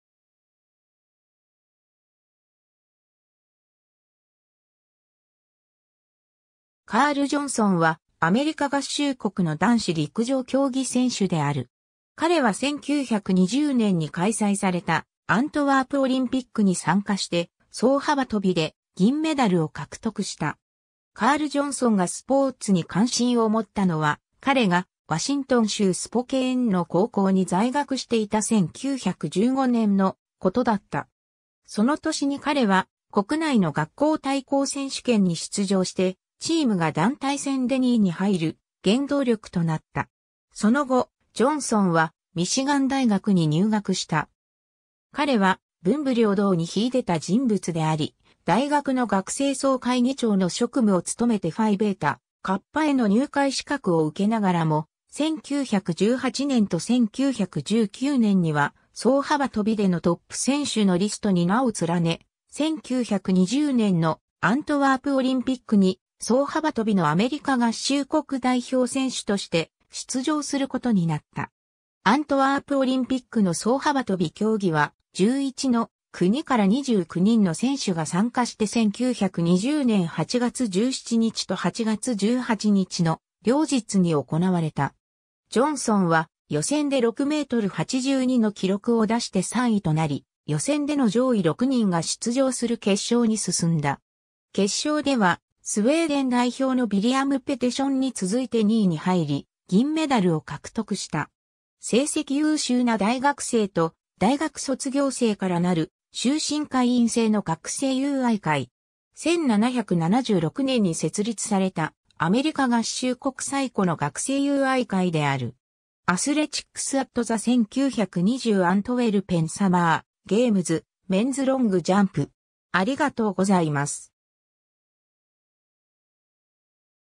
カール・ジョンソン (陸上選手)
カール・ジョンソン（Carl Edward Johnson、1898年5月21日 - 1932年9月13日）は、アメリカ合衆国の男子陸上競技選手である。
彼は1920年に開催されたアントワープオリンピックに参加して、走幅跳で銀メダルを獲得した。
 カール・ジョンソンがスポーツに関心を持ったのは、彼がワシントン州スポケーンの高校に在学していた1915年のことだった。
その年に彼は国内の学校対抗選手権に出場して、チームが団体戦で2位に入る原動力となった。
 その後、ジョンソンはミシガン大学に入学した。
彼は文武両道に秀でた人物であり、大学の学生総会議長の職務を務めてファイ・ベータ・カッパ（en:Phi Beta Kappa Society） への入会資格を受けながらも、1918年と1919年には走幅跳でのトップ選手のリストに名を連ね、1920年のアントワープオリンピックに走幅跳のアメリカ合衆国代表選手として出場することになった。
 アントワープオリンピックの走幅跳競技は、11の国から29人の選手が参加して1920年8月17日と8月18日の両日に行われた。
ジョンソンは予選で6メートル82の記録を出して3位となり、予選での上位6人が出場する決勝に進んだ。
決勝ではスウェーデン代表のヴィリアム・ペテションに続いて2位に入り、銀メダルを獲得した。
 ^ 成績優秀な大学生と大学卒業生から成る終身会員制の学生友愛会。
1776年に設立されたアメリカ合衆国最古の学生友愛会である。
 ^ Athletics at the 1920 Antwerpen Summer Games:Men's Long JumpArchived 2020年4月17日, at the Wayback Machine.（英語）